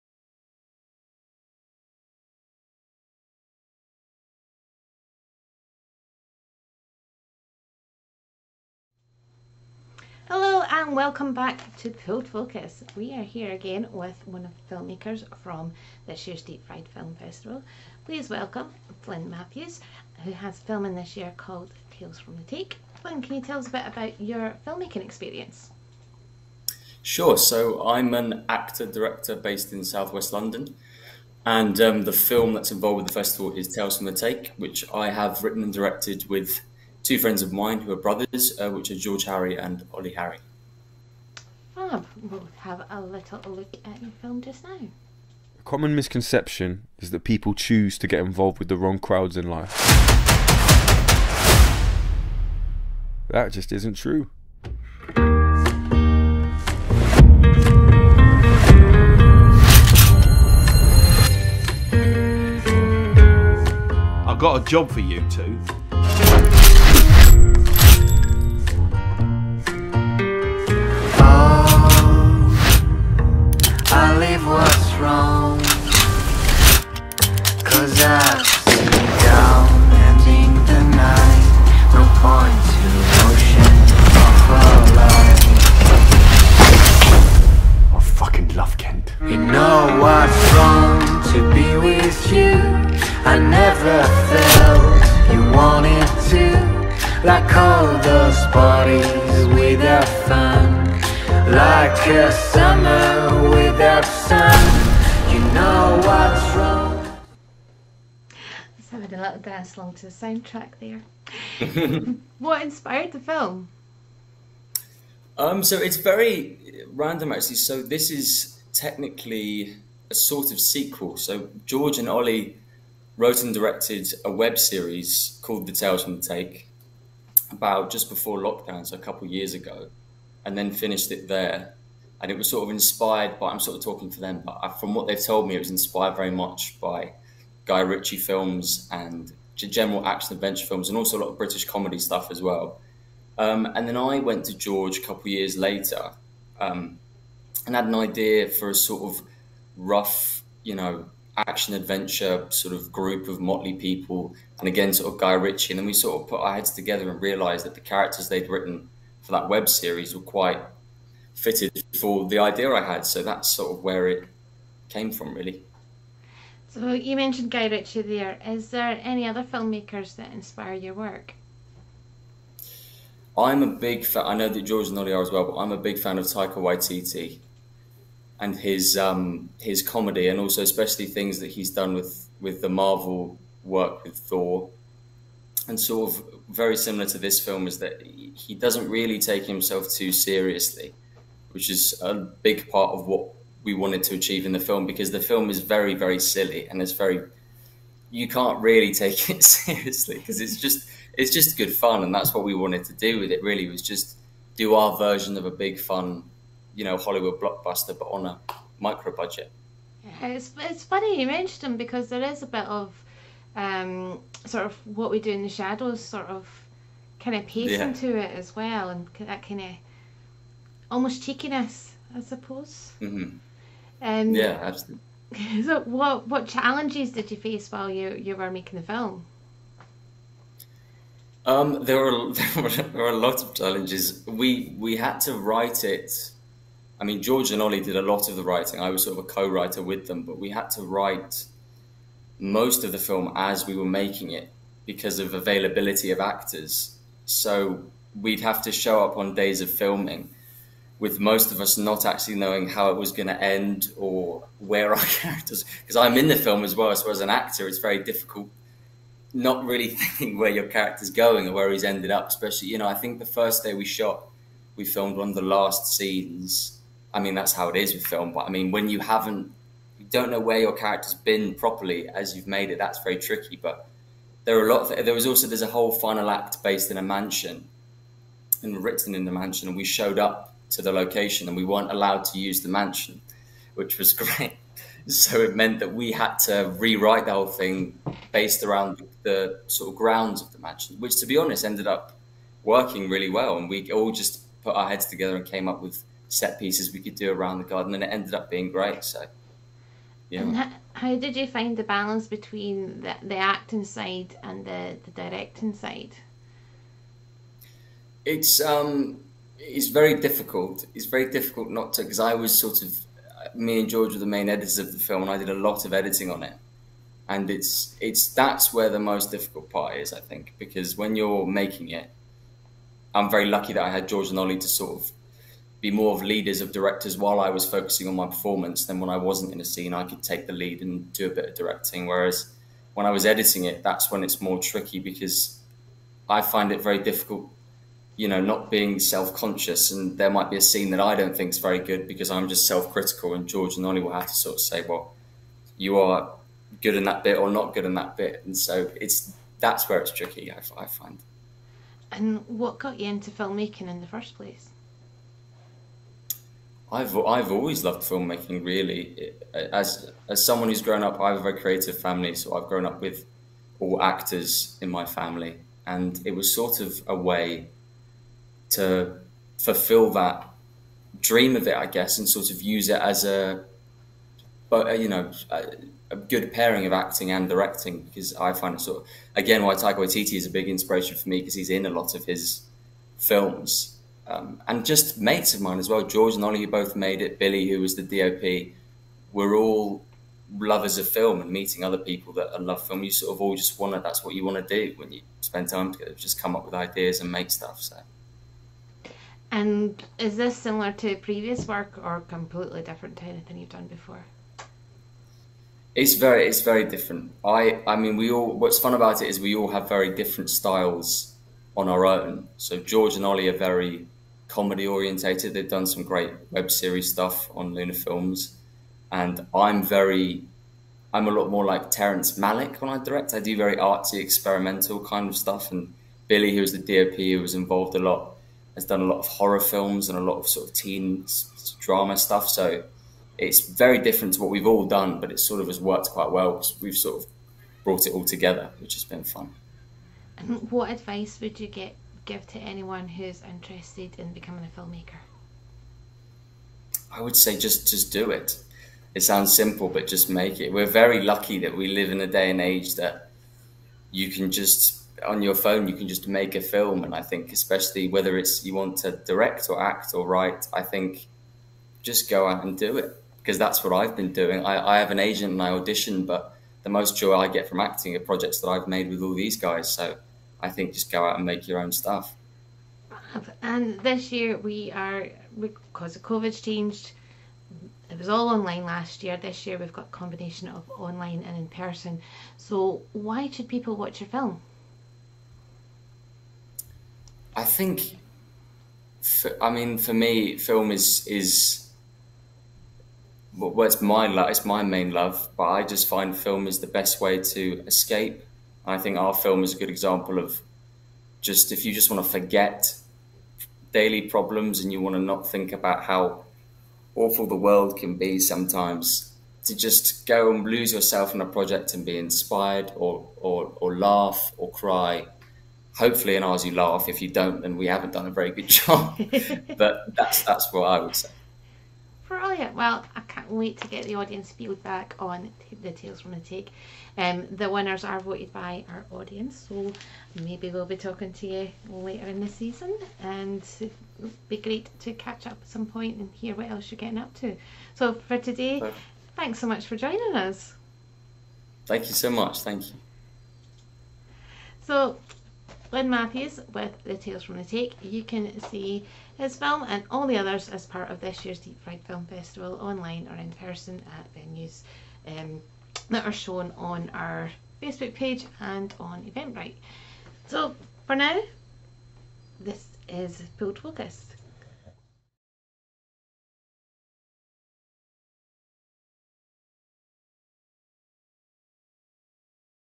The Tales From The Take - Deep Fried Film Festival 2022
Welcome back to Pulled Focus where we meet Flynn Mathews, Director of The Tales From The Take.